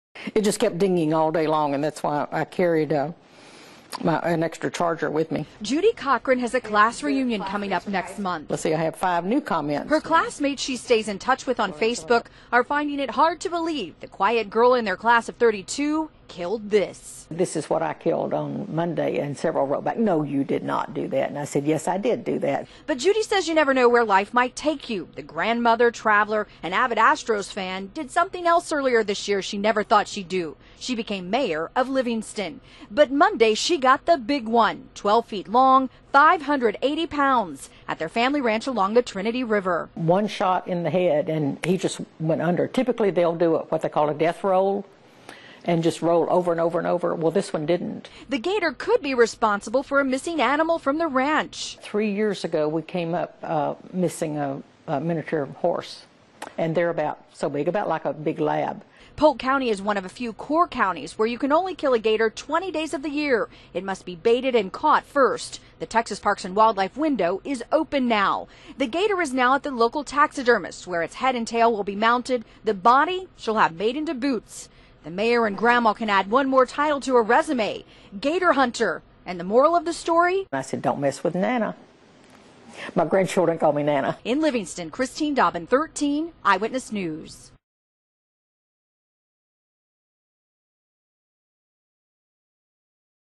'Don't mess with Nana!' - Grandma kills gator
A Texas mayor and grandmother surprised many by shooting and killing a 12-foot long alligator.